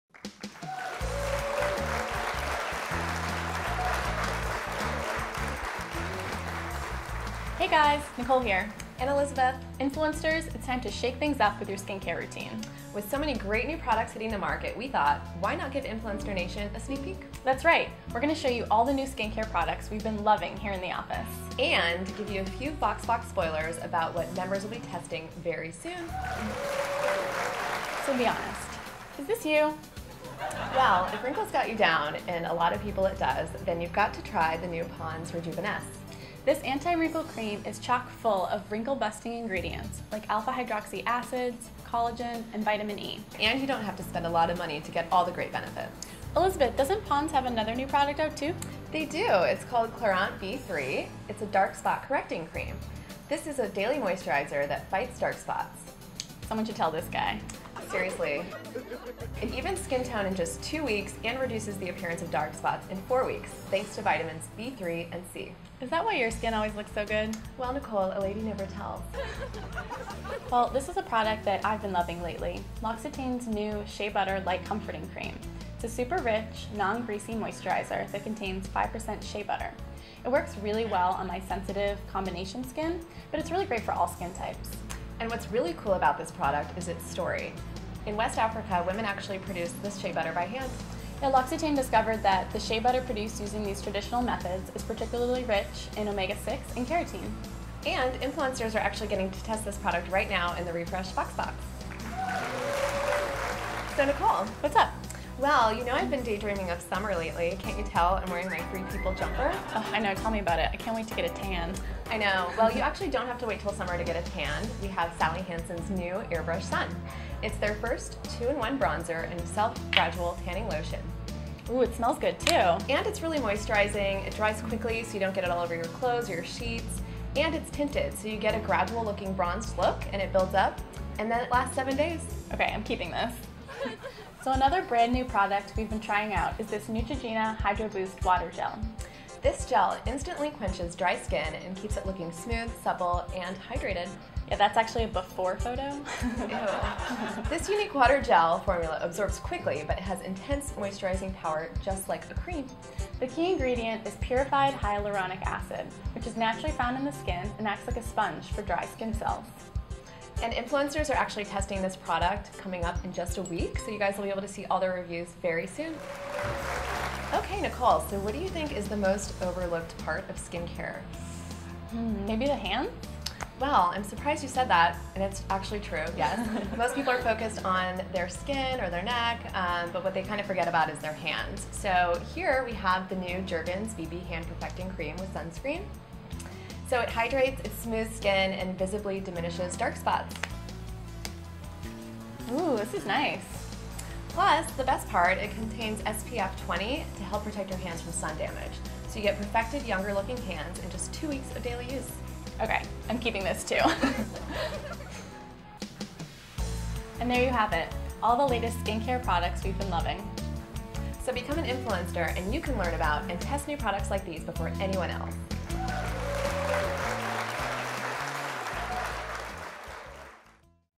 SPOILER ALERT: BIG reveal of new skincare products Influensters will test next!
Are you ready to take your skincare routine to the next level? Check out these amazing brand new products hitting the market, courtesy of some of our favorite brands. You never know, you could be testing one of these products in an upcoming VoxBox!

Want to be in the running to test these products?

Want to learn more about the products featured? Read on below!

These products will be sent to a few lucky Influensters complimentary for testing purposes.

Elizabeth is featured wearing the Drippy Mesh Collar Necklace and Firefly Jumpsuit  from Free People.

Video Production by Josh Allen Images